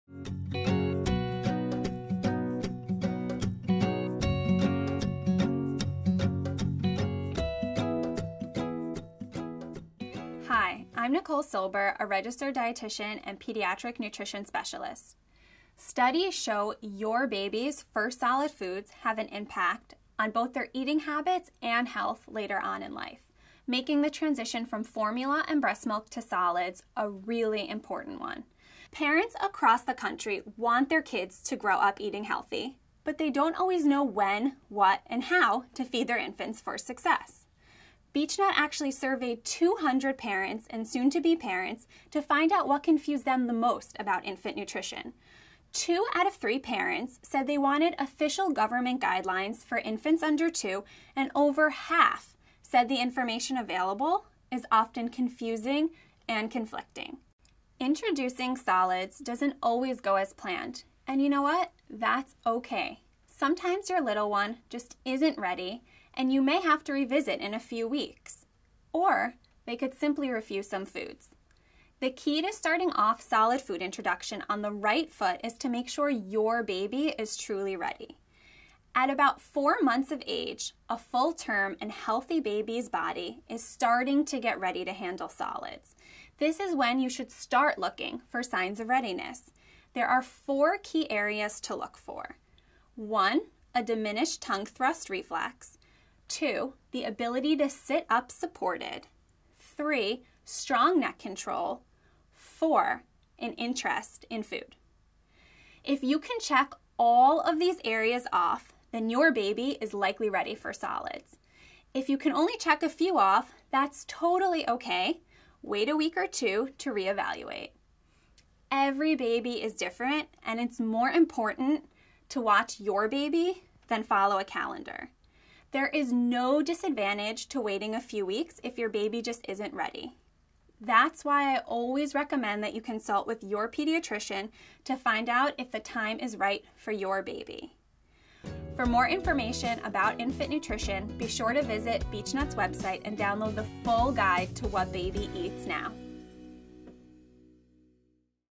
What Baby Eats Now - Introducing Solids
We partnered with Nicole Silber, RD, CSP, CDN and pediatric nutrition specialist to provide advice on when to introduce solids, the signs of readiness to look for, what to do if your baby isn’t quite ready for solids yet.